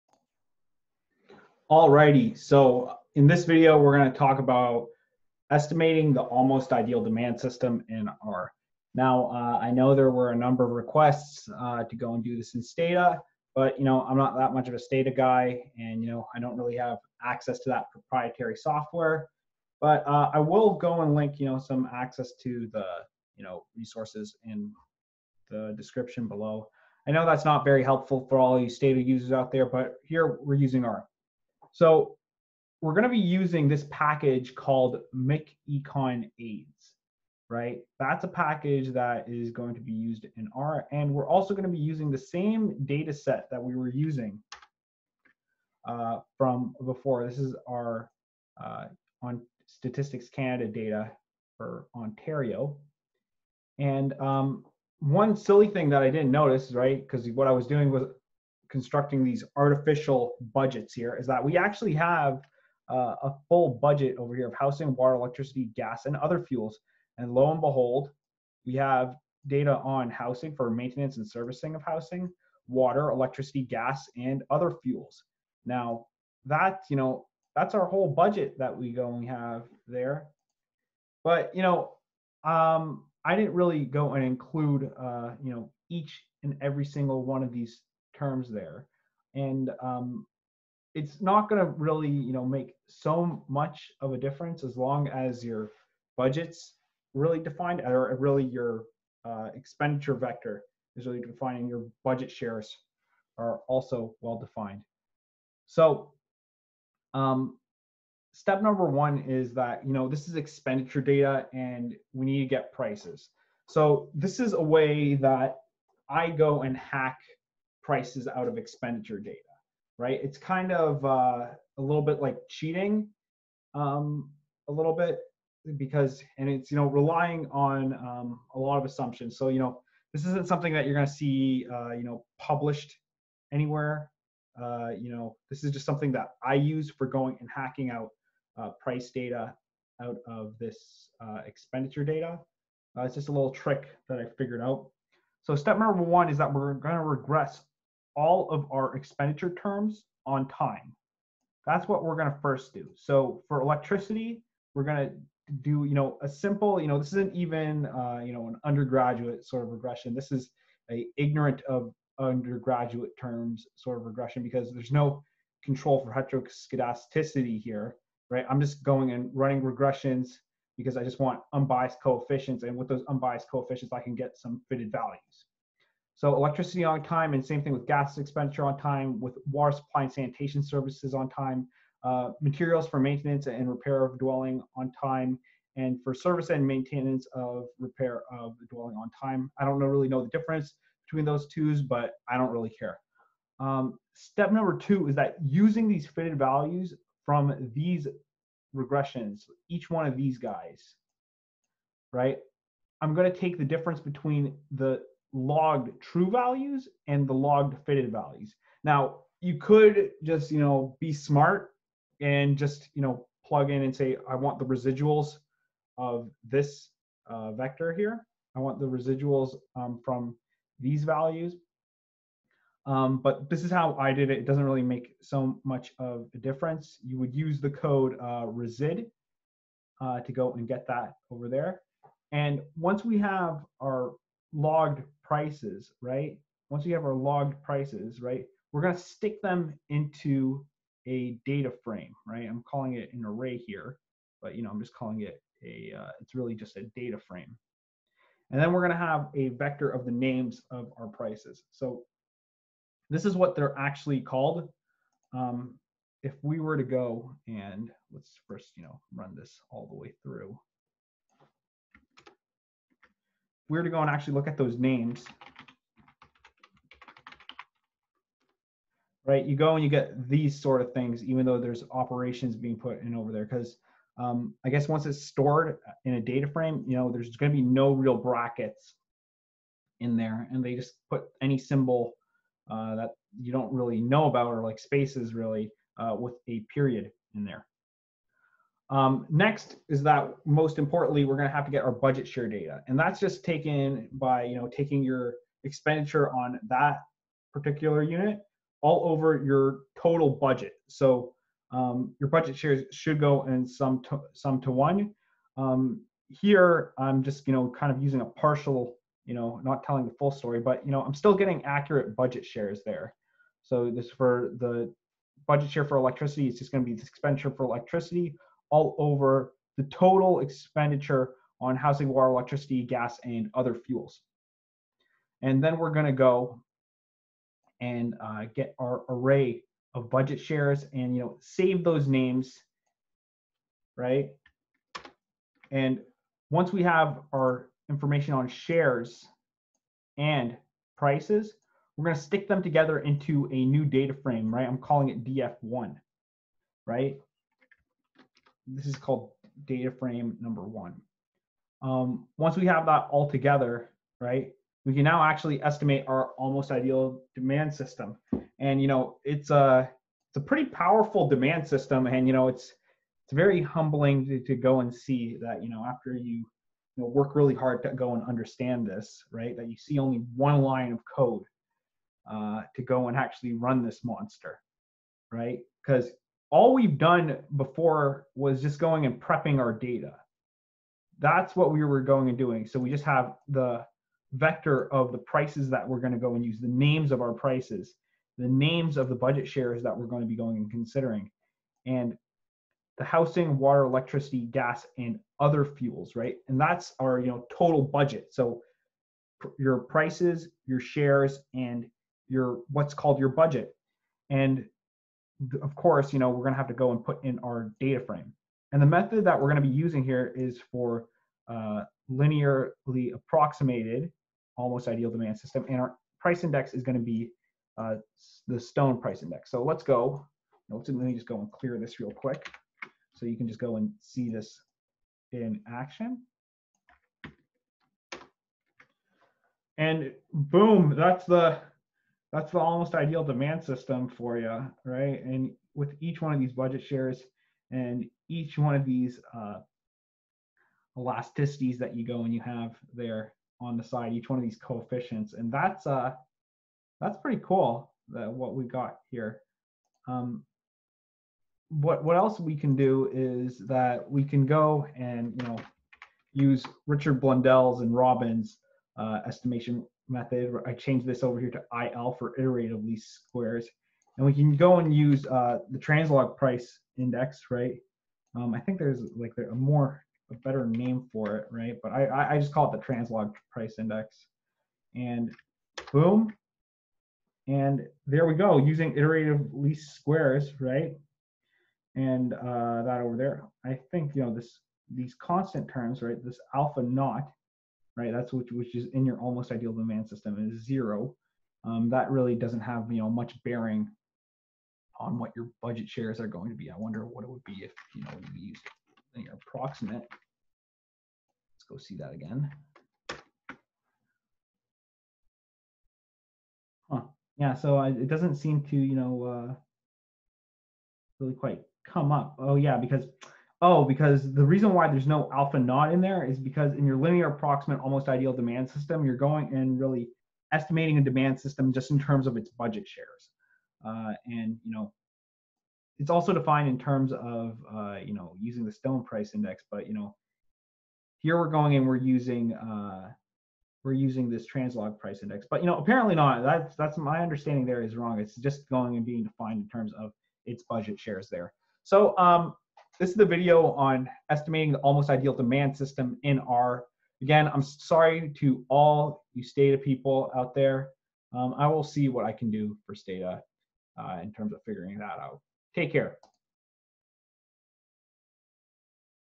Estimating the Almost Ideal Demand System in R
In this video I go through how I estimate the almost ideal demand system in R using expenditure data. I show off a trick I use for getting data on prices out of expenditure data and demonstrate how after cleaning up your data you can estimate this demand system in just one line!